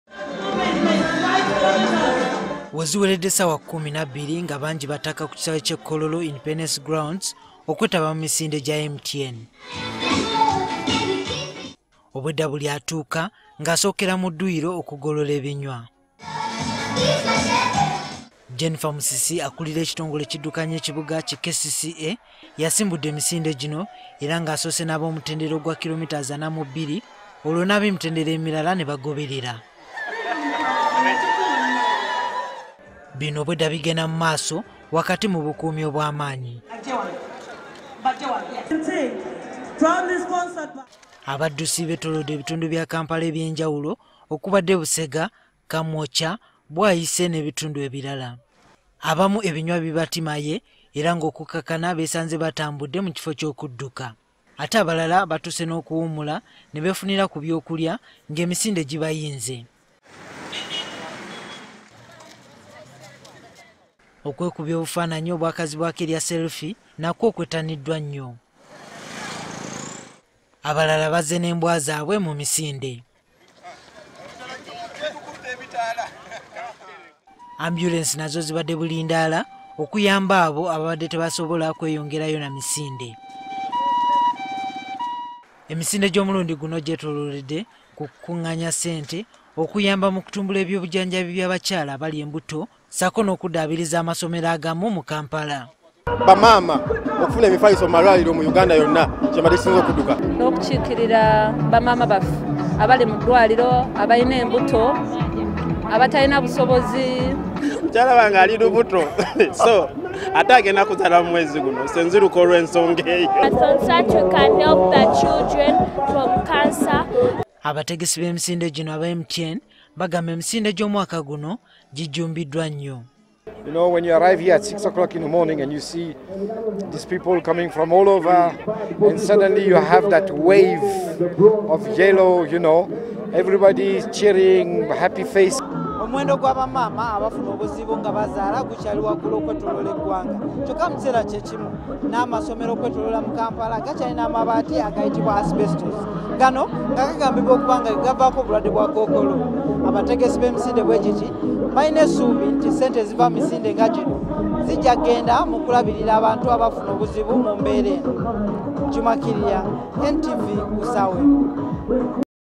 Emisinde gya MTN: Egy’omwaka guno gijjumbiddwa nnyo abantu
Emisinde gya MTN egy’omwaka guno egyategekeddwa okudduukirira eby’obujjanjabi by’abakyala abali embuto n’okuddaabiriza amasomero gijjumbiddwa nnamungi w’omuntu